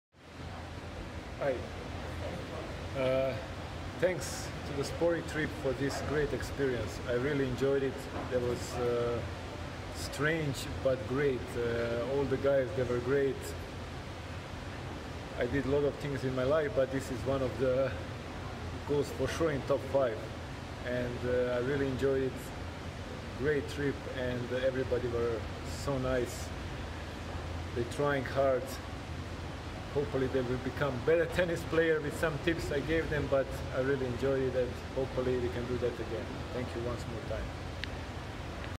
Goran Ivanišević - Thoughts on Tennis Clinic with Sportytrip
Our work brings us the good fortune of interacting with some of the best athletes from across the globe. Great experience to have had 2001 Wimbledon winner Goran Ivanišević tutor our clients in a special tennis clinic that was held in Melbourne! 🎾